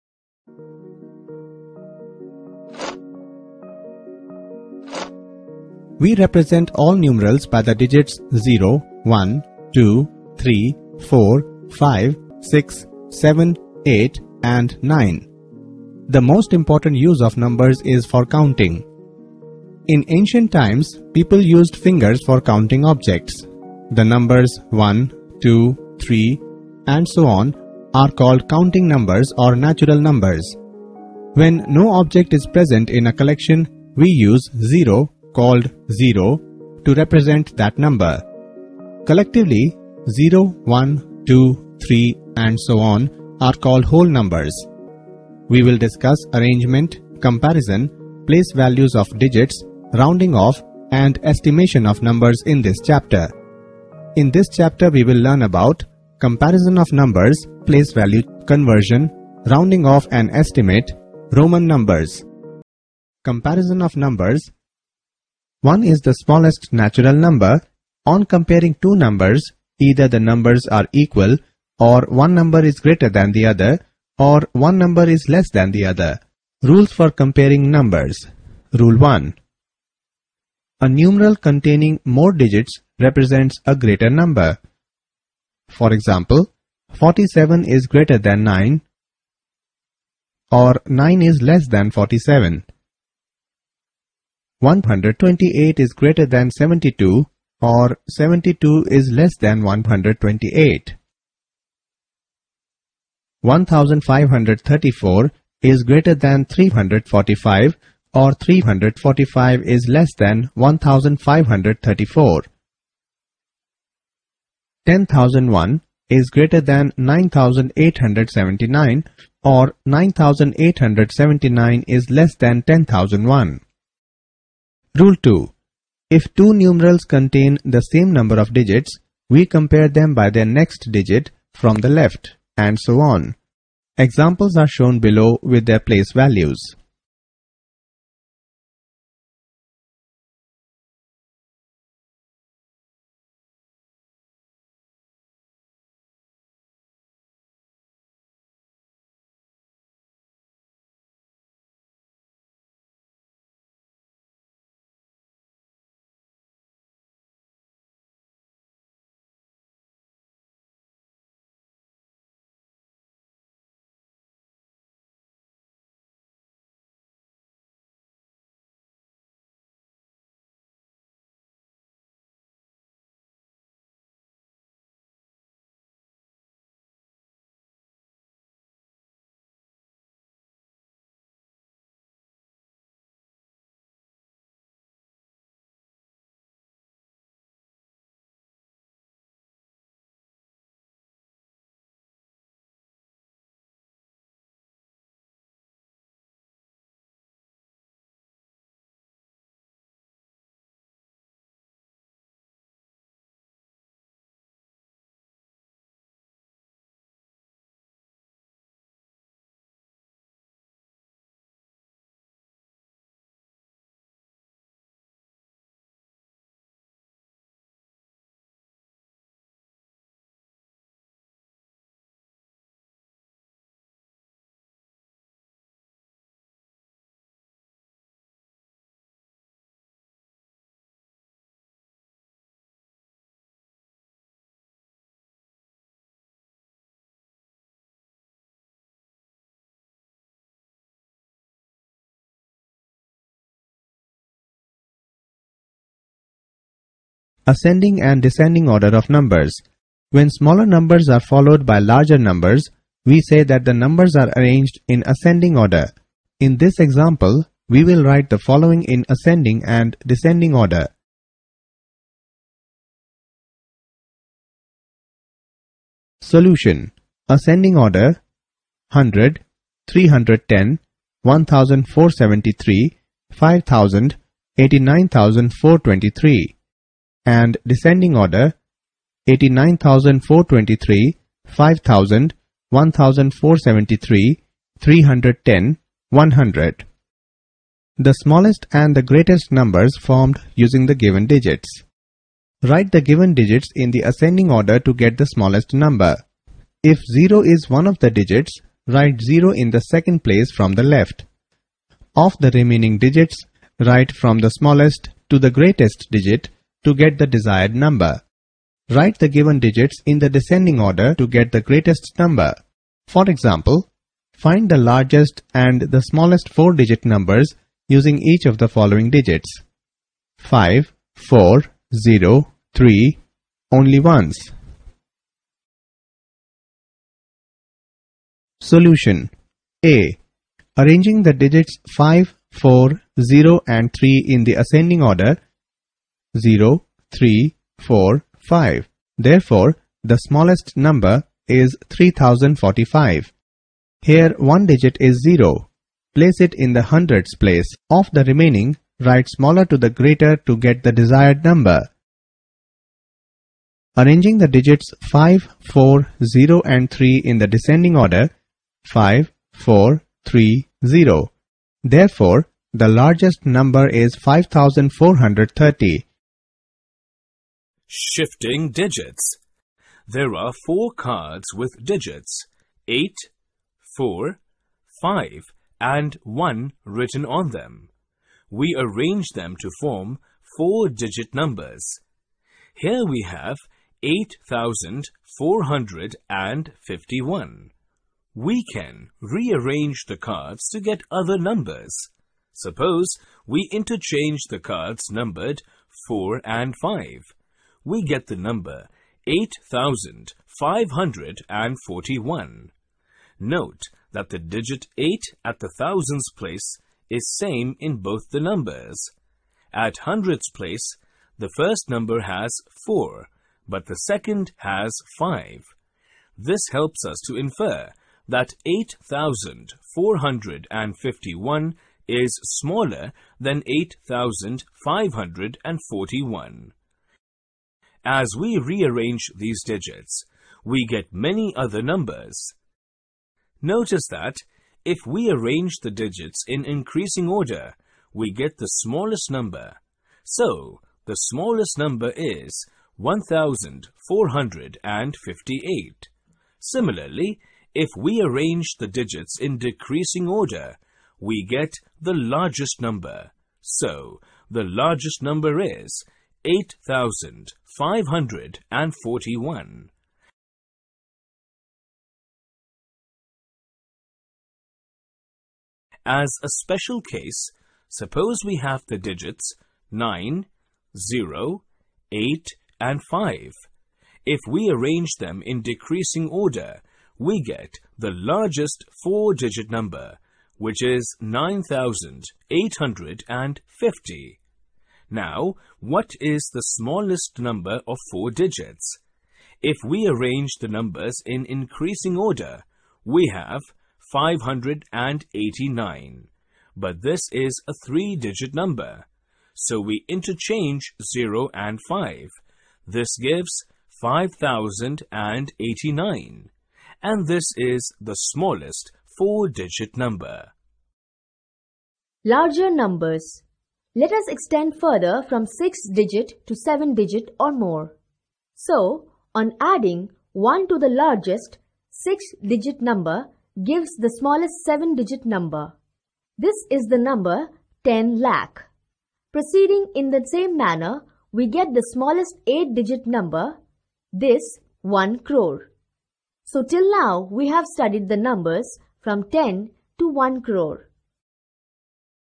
Knowing our Numbers | Comparison of numbers | Class 6
We represent all numerals by the digits 0,1, 2, 3, 4, 5, 6, 7, 8, and 9. The most important use of numbers is for counting. In ancient times, people used fingers for counting objects. The numbers 1, 2, 3, . . . are called counting numbers or natural numbers. When no object is present in a collection we use '0' called zero to represent that number. Collectively, 0, 1, 2, 3 ...are called whole numbers.We will discuss arrangement, comparison, place values of digits, rounding off and estimation of numbers in this chapter.